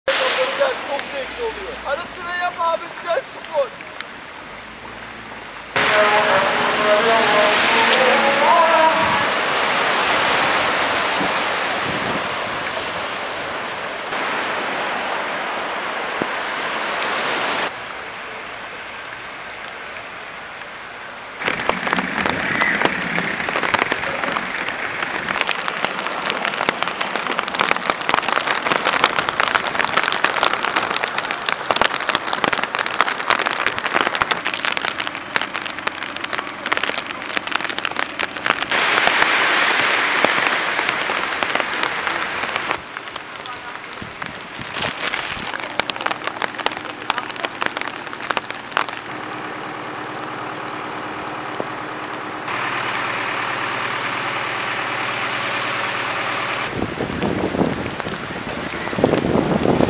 Giren bir daha çıkamadı. İşte sağanak yağış ve Karasu Sakarya
Kuvvetli sağanak yağış Karasu'yu da vurdu. Ankara caddesi üzerinde birçok otomobil yolda mahsur kalırken, oto kurtarıcılar mesaideydi. Bazı vatandaşlar da araçlarını iterek yol kenarına çekerken, otomatik vites olanlar ise yerinden kımıldamadı. İşte o anlar.